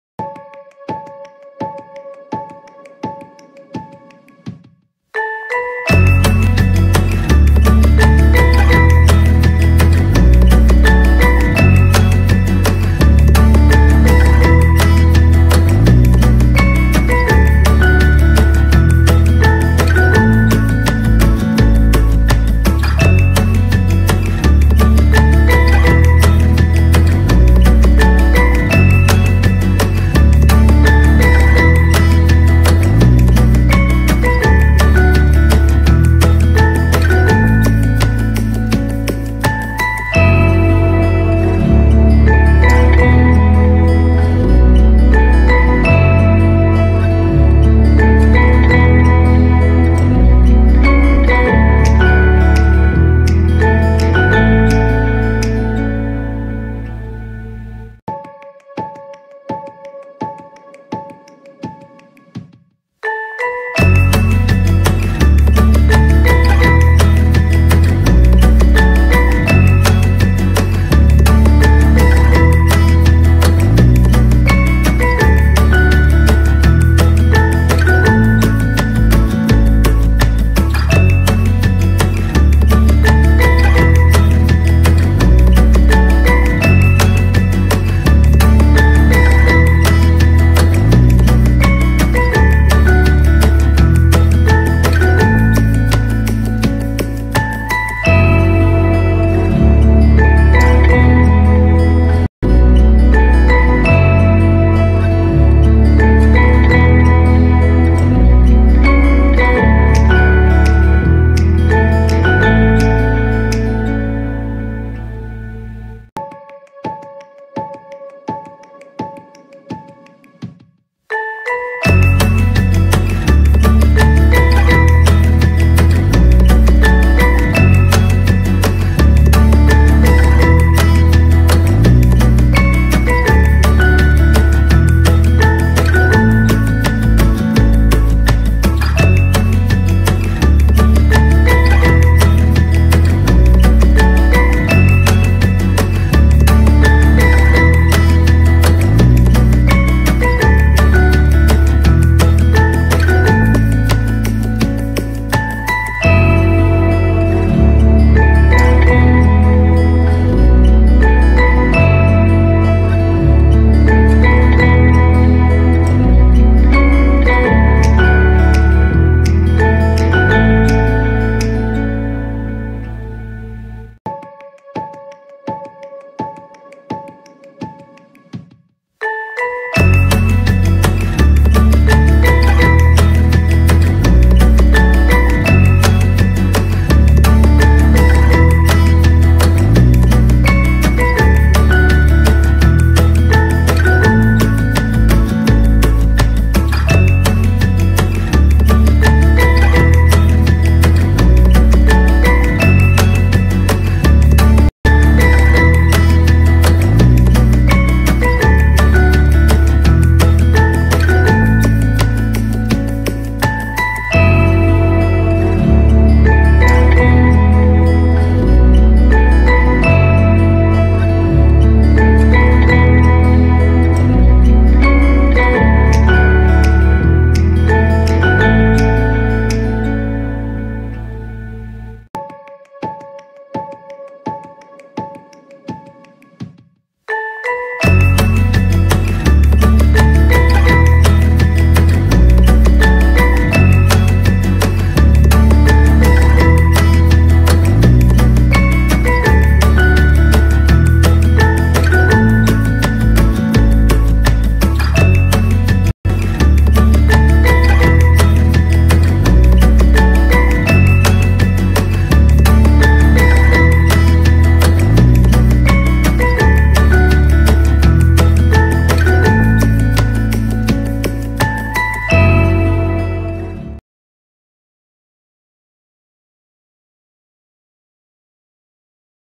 Realistic Drawing Tutorial | How to Draw Utensils
2HB led pencil and black pencil color is used in this drawing.
Hope you like this Realistic Utensils Drawing.